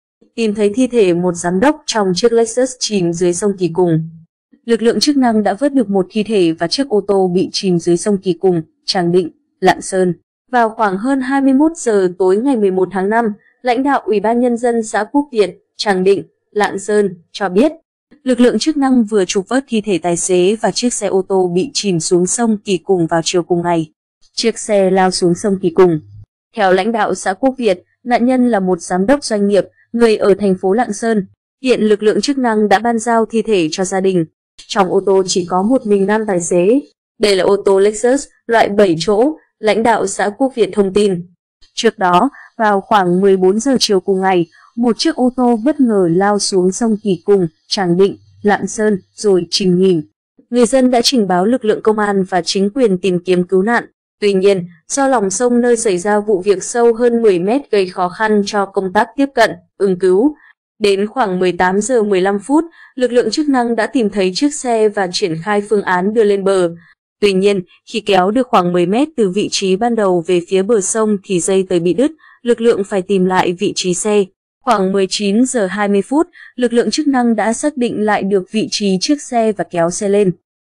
Tìm thấy thi thể một giám đốc trong chiếc Lexus chìm dưới sông Kỳ Cùng
Lực lượng chức năng đã vớt được một thi thể và chiếc ô tô bị chìm dưới sông Kỳ Cùng (Tràng Định, Lạng Sơn).Đến khoảng 18h15, lực lượng chức năng đã tìm thấy chiếc xe và triển khai phương án đưa lên bờ. Tuy nhiên, khi kéo được khoảng 10m từ vị trí ban đầu về phía bờ sông thì dây tời bị đứt, lực lượng phải tìm lại vị trí xe. Khoảng 19h20, lực lượng chức năng đã xác định lại được vị trí chiếc xe và kéo xe lên.
Người dân đã trình báo lực lượng công an và chính quyền tìm kiếm cứu nạn. Tuy nhiên, do lòng sông nơi xảy ra vụ việc sâu hơn 10m gây khó khăn cho công tác tiếp cận, ứng cứu.
“Trong ô tô chỉ có một mình nam tài xế. Đây là ô tô Lexus, loại 7 chỗ”, lãnh đạo xã Quốc Việt thông tin.
Chiếc xe lao xuống sông Kỳ Cùng
Trước đó, vào khoảng 14h chiều cùng ngày, một chiếc ô tô bất ngờ lao xuống sông Kỳ Cùng (Tràng Định, Lạng Sơn) rồi chìm nghỉm.
Theo lãnh đạo xã Quốc Việt, nạn nhân là một giám đốc doanh nghiệp, người ở TP.Lạng Sơn. Hiện lực lượng chức năng đã ban giao thi thể cho gia đình.
Vào khoảng hơn 21h tối 11/5, lãnh đạo UBND xã Quốc Việt (Tràng Định, Lạng Sơn) cho biết, lực lượng chức năng vừa trục vớt thi thể tài xế và chiếc xe ô tô bị chìm xuống sông Kỳ Cùng vào chiều cùng ngày.